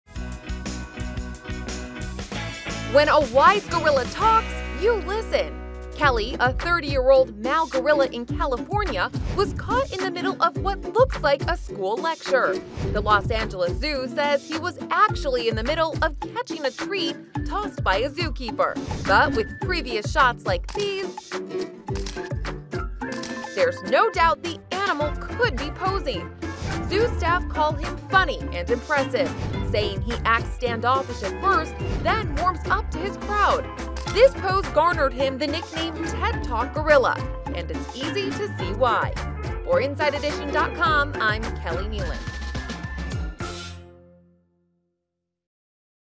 Why This Is The Smartest Gorilla You'll Ever Meet
Another gorilla is becoming an internet sensation after the primate was photographed giving what appears to be a lecture to a crowd. "Kelly is a funny boy, big and impressive," zookeeper Nancy Bunn said. "He acts standoffish at first, then warms up to a crowd." The photo was published as a promotional image for a summer event at the Los Angeles Zoo, but the ape quickly garnered the nickname, "TED Talk Gorilla." InsideEdition.com's Keleigh Nealon has more.